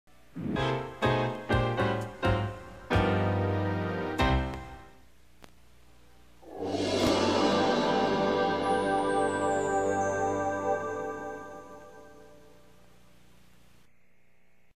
The Preston/Stephen/Fischer Company/World International Network logos (1992)
Reupload December 27th 2012 the video. The World International Network Film closer in the screen logo was still useing animation.